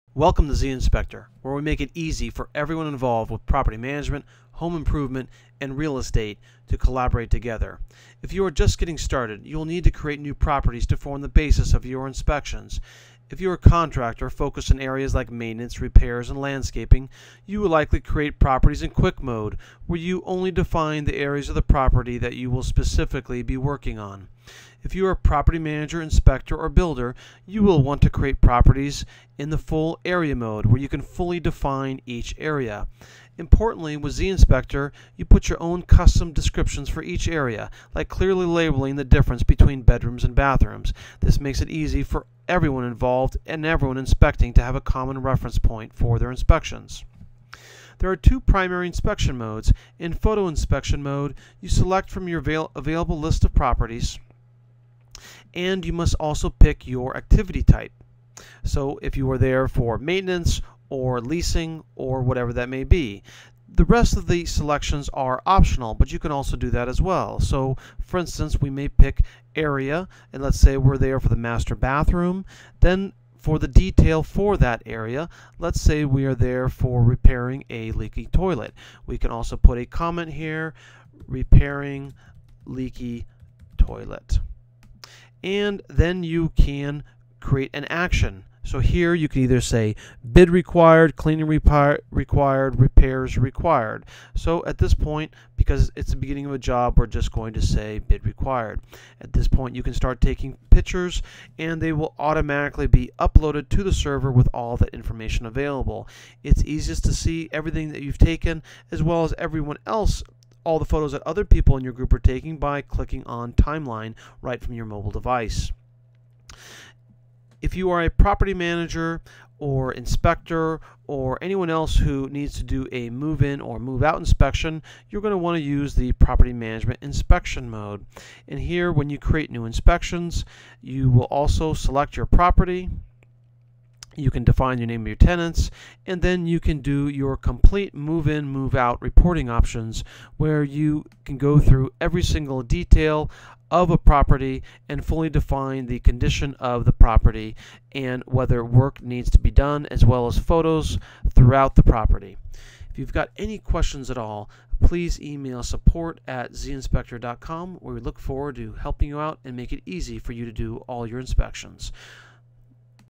zInspector - Getting Started with the Mobile App
zInspector is a complete real estate, property management, and home improvement collaboration tool. zInspector enables all of your employees inspection notes and photos to be accessible, secured, and backed up.